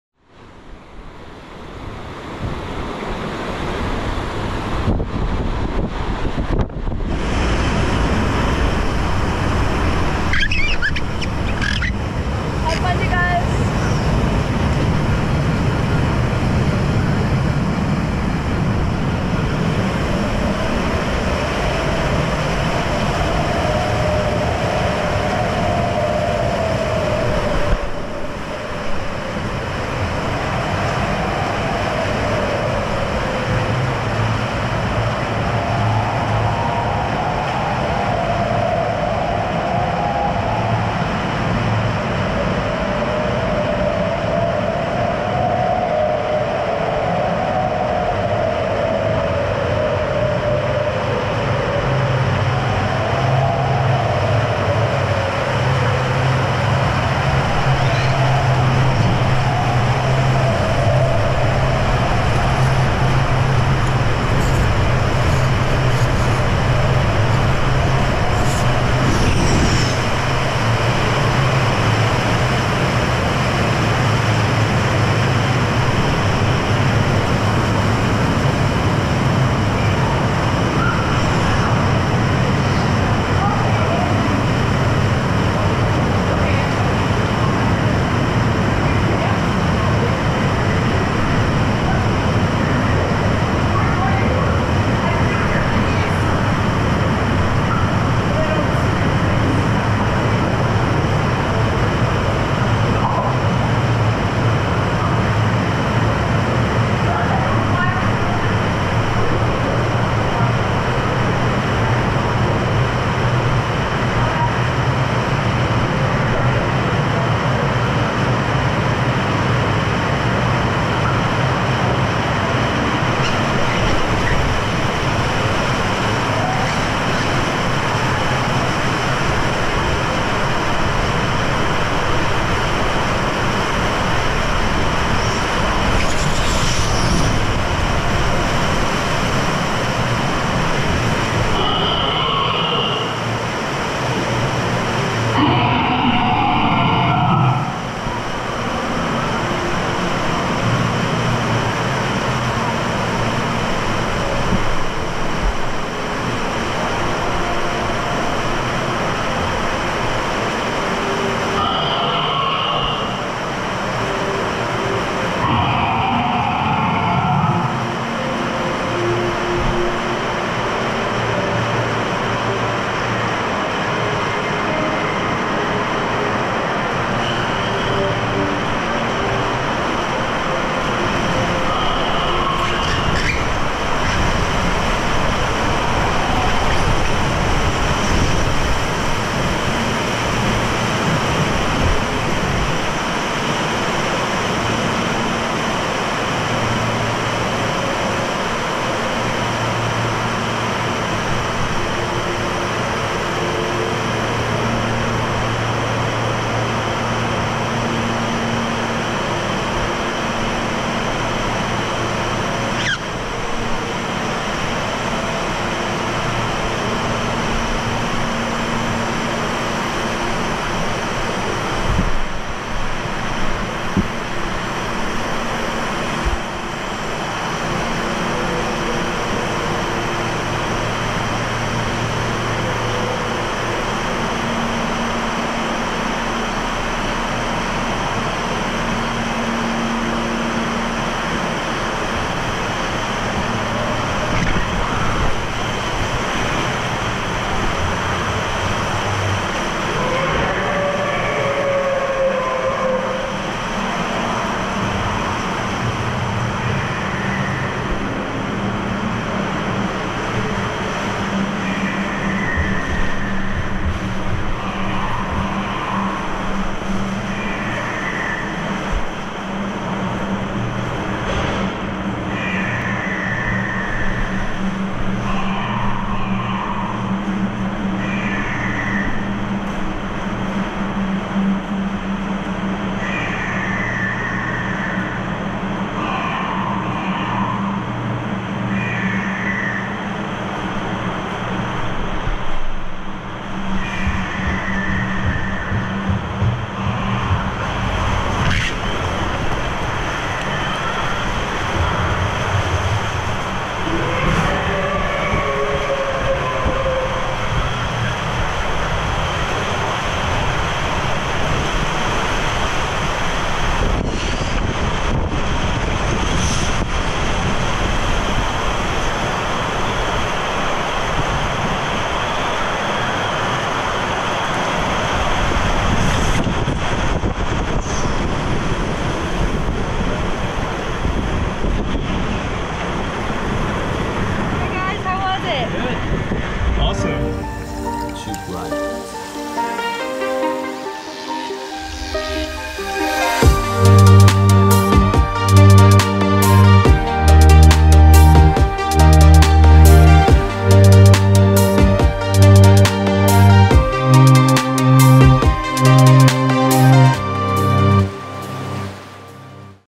THEMED Water Park Ride! Voyage to the Center of the Earth - Water World
Best Low Light POV!

This is the Voyage to the Center of the Earth water ride at Water World in Federal Heights, Colorado, United States! The attraction is a unique combination of a theme park dark ride and a water slide. Before the ride, guests have to pick up a round-raft from the runout of the slide and carry it to the top of the hill where the entrance is located. Those who purchased the Tube Valet wristband can directly proceed to the start of the ride to pick up their tube. After embarking the raft, guests float through a dark Lazy River part before suddenly zooming through a steep helix which really caught us by surprise! The entire ride is fitted with animatronics, light effects and a lot of other stuff you would never expect to find in a water slide - not even Disney or Universal ever made a slide like that. At the end of the ride, guests plunge down a steep drop before ending their ride in a shallow runout lane. Voyage is a one-of-a-kind water attraction which truly lives up to the hype. One of the best water slides we have ever tried, especially when it comes to the theming.